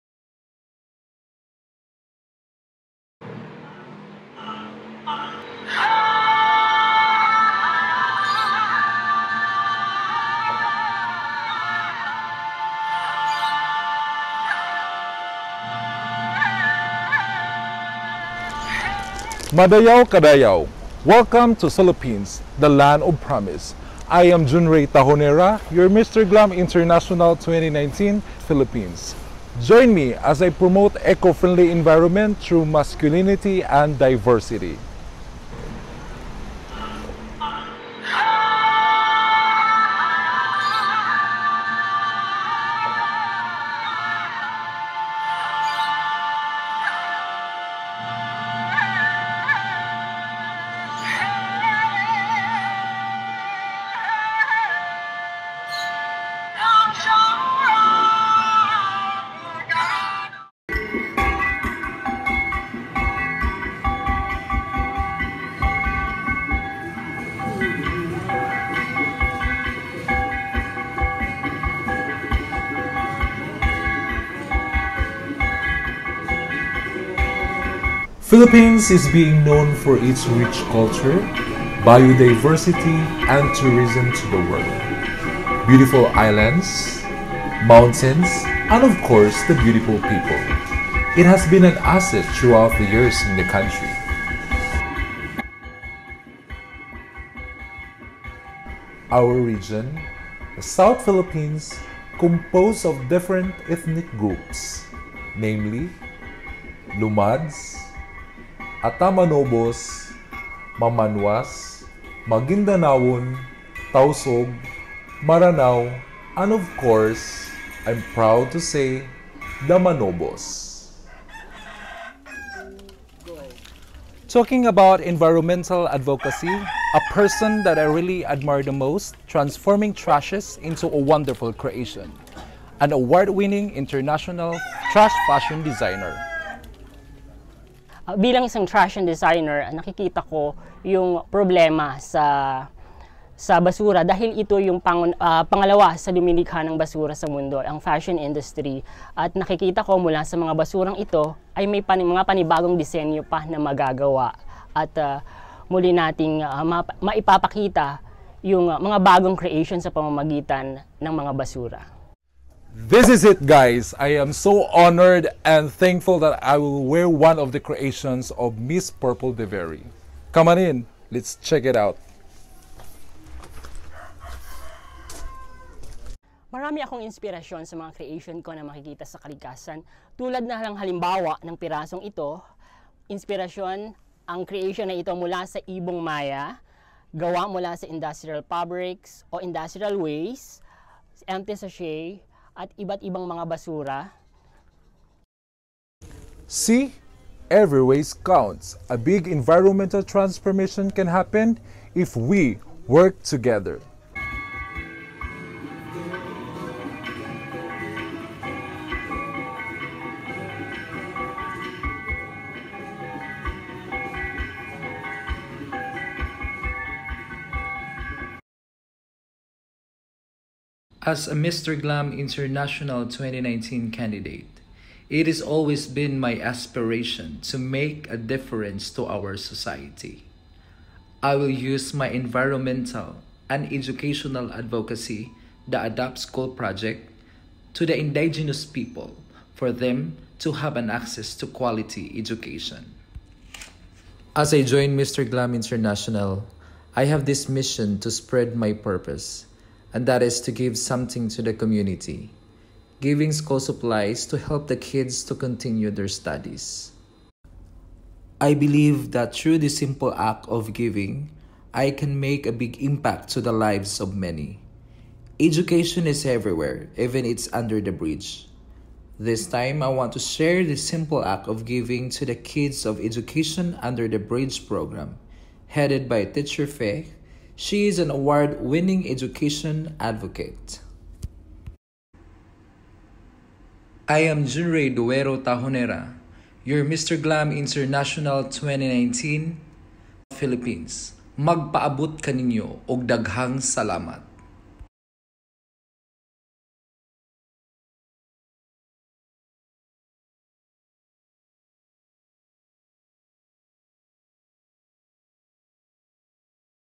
MR. GLAM INTERNATIONAL - PHILIPPINES INTRO VIDEO
Advocacies and Introduction of Mr. Glam International - Philippines for the Upcoming Mr Glam International